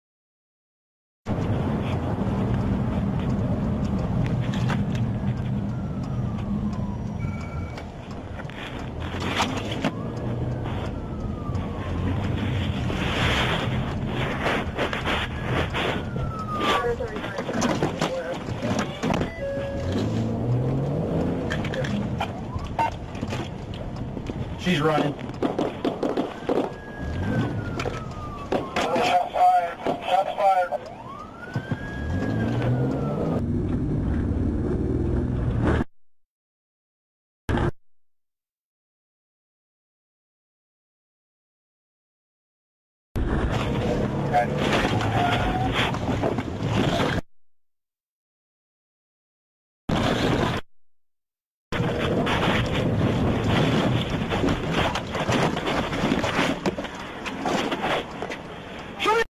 Madison Dickson, a wanted suspect, is seen on video getting ran over by police.
Madison Dickson, a wanted criminal, was ran over during a gunfight with officers, according to Tulsa Police. 18th of March